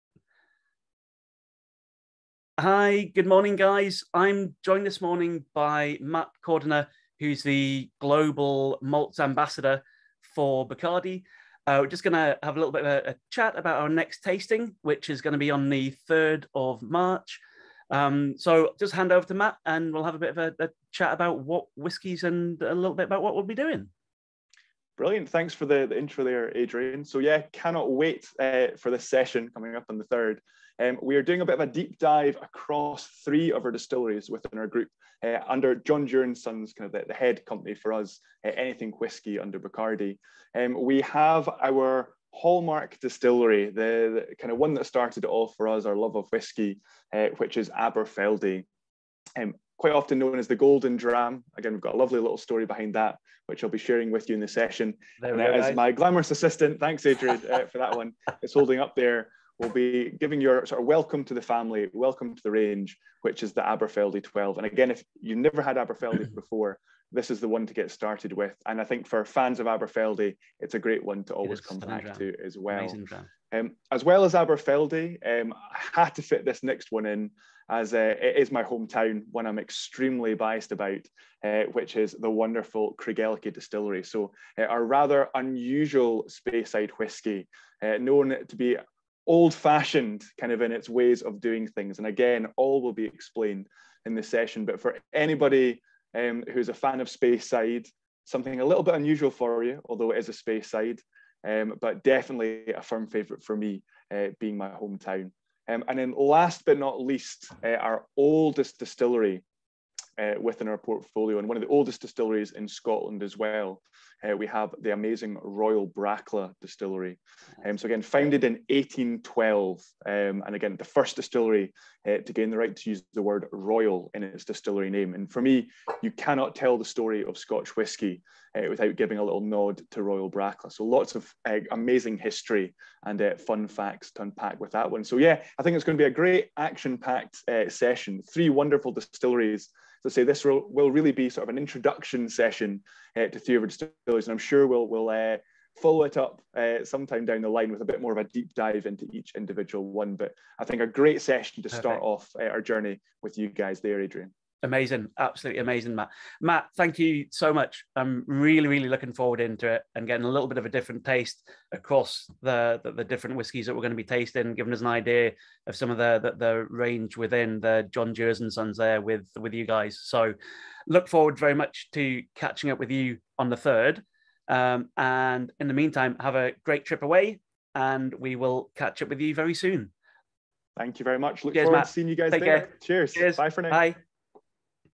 John Dewars Tasting Introduction
Whisky Mastery tasting introduction with Global Malts Ambassador Matt Cordiner.